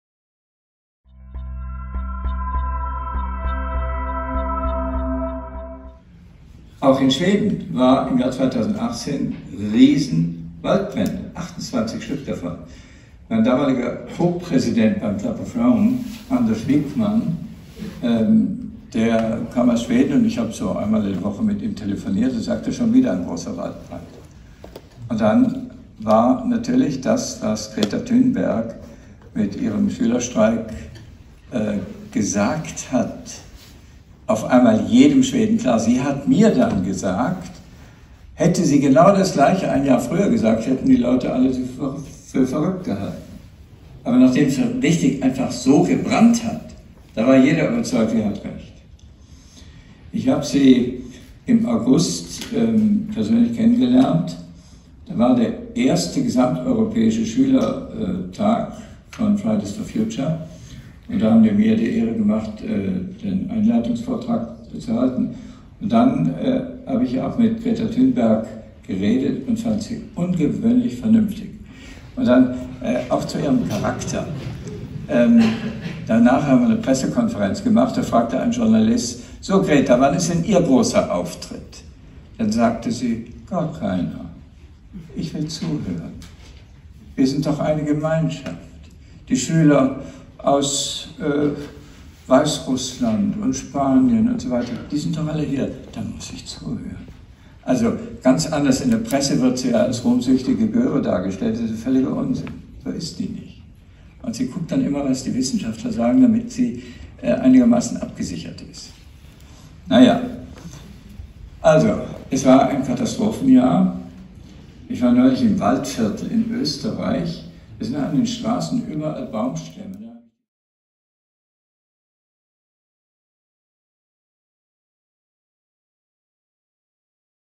Ernst Ulrich von Weizsäcker über Greta Thunberg
Während eines Vortrages in der Handelskammer Hamburg spricht Ernst Ulrich von Weizsäcker kurz über Greta Thunberg.